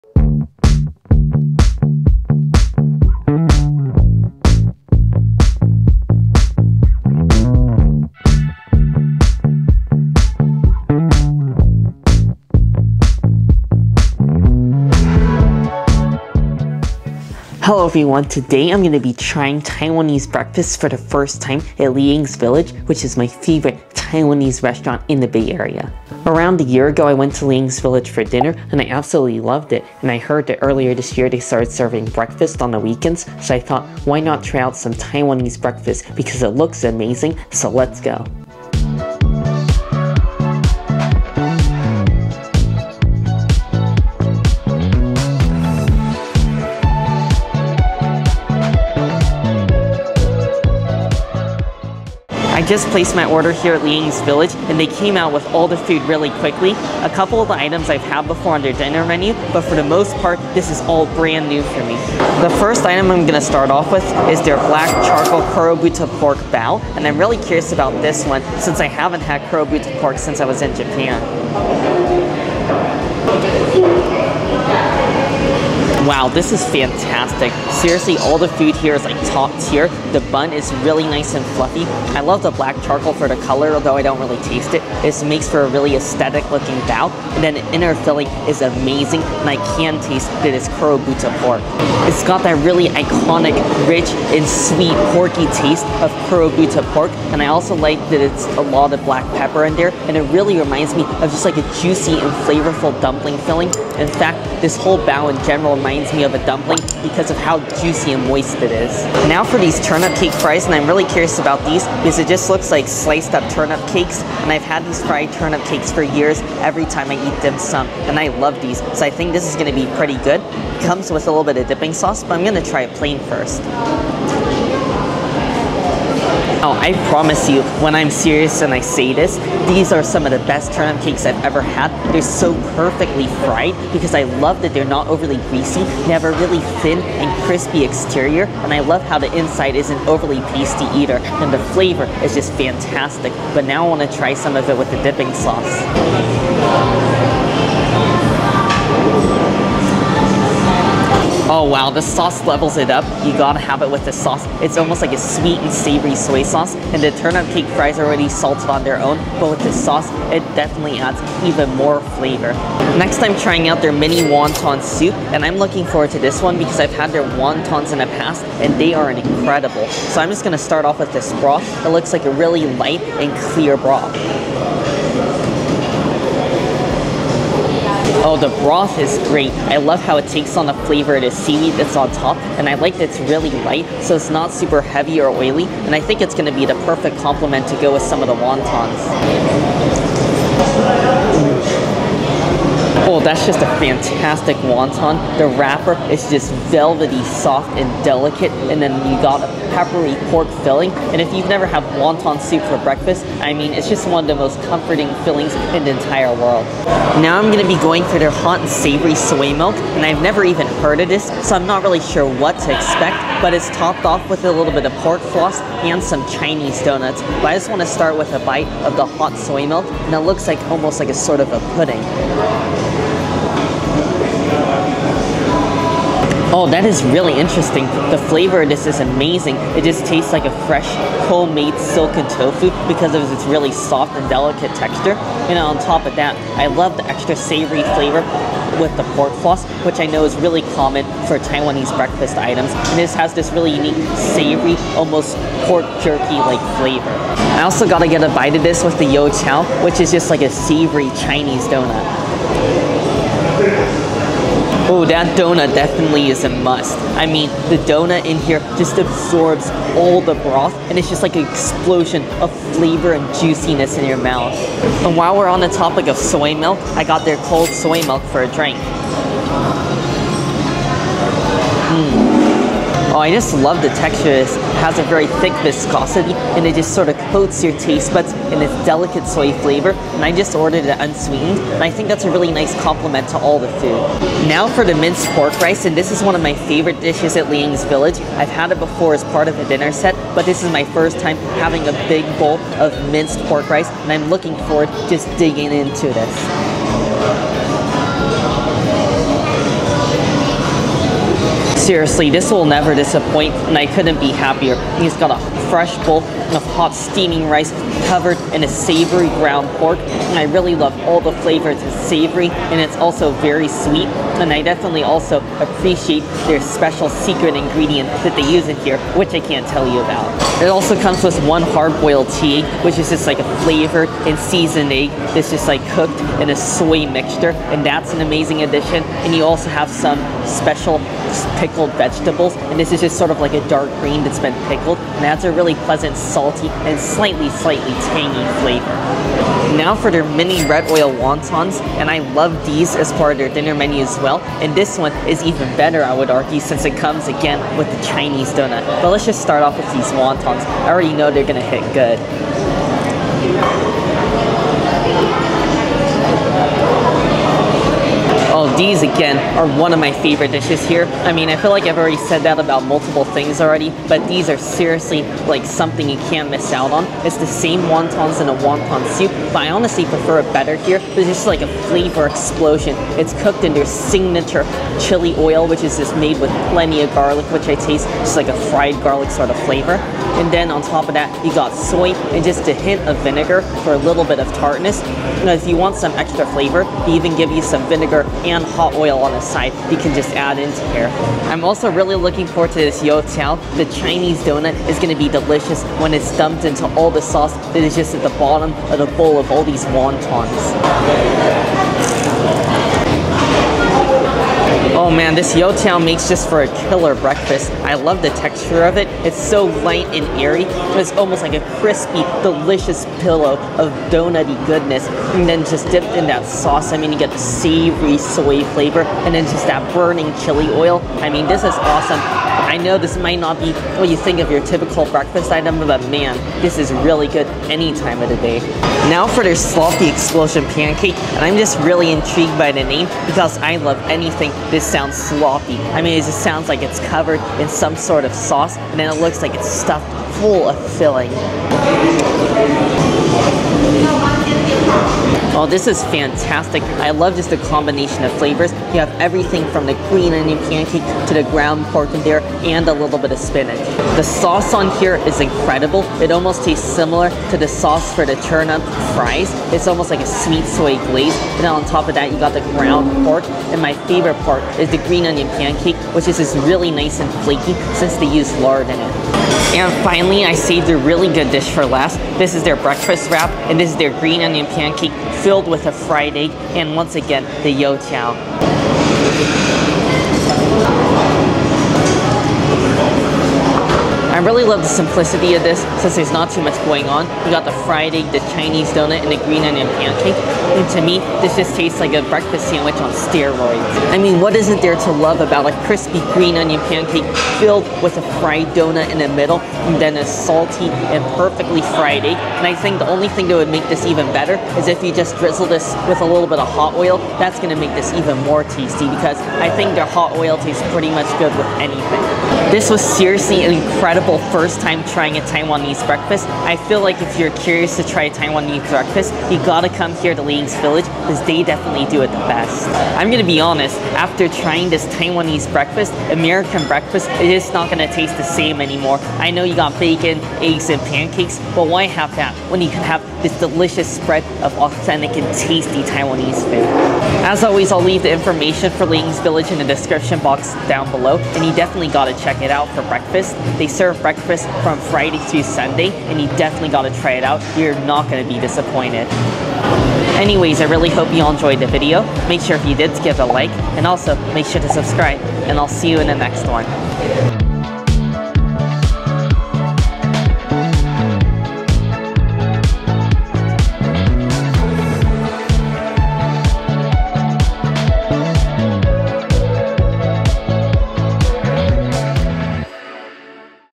FIRST TIME Trying Taiwanese Breakfast | BEST Taiwanese Breakfast In The Bay Area At Liangs Village!
In today’s video I try Taiwanese breakfast for the first time at my favorite Taiwanese restaurant, Liangs Village in Cupertino.   This was a fantastic meal and you should all try it yourselves!  Note: breakfast served Friday - Sunday 8am - 11am.